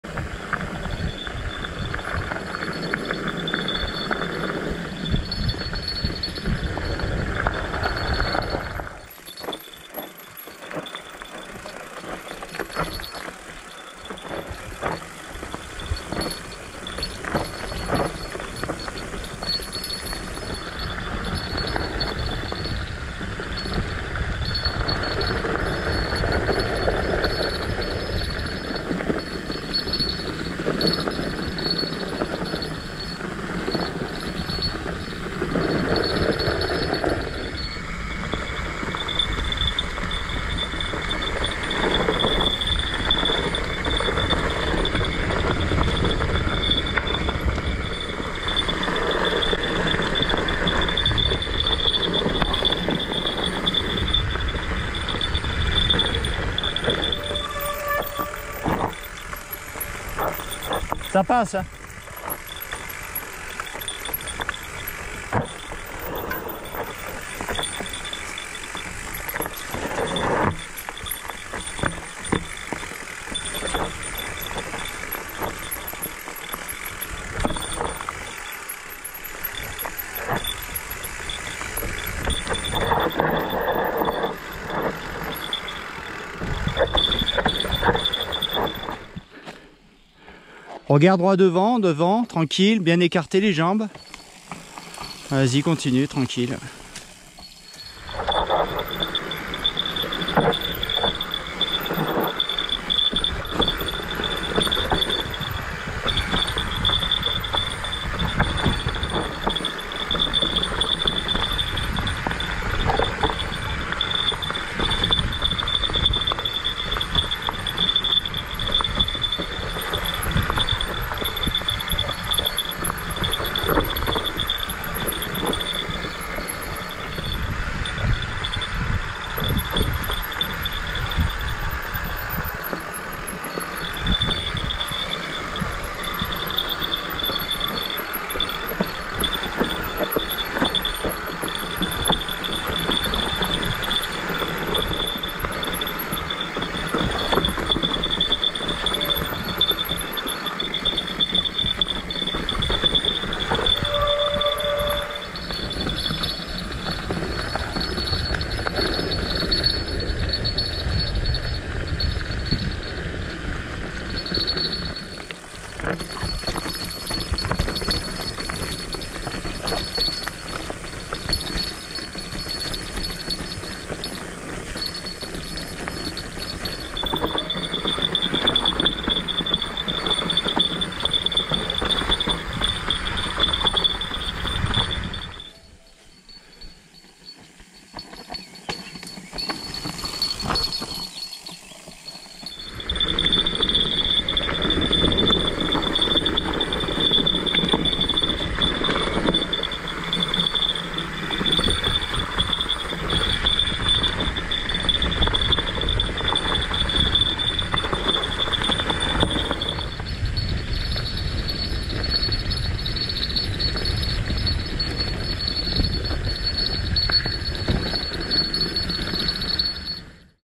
Descente Enduro à Cazavet en Ariège (parcours balisé n°8 Espace VTT Couserans)
C'est la descente enduro de Cazavet, très très ludique et facilement accessible par piste (9 km et 400m de D+ en suivant le parcours n°9 au départ de Cazavet)
Une fois n'est pas coutume, pas de musique, seulement la descente au rythme des clochettes de VTT !!!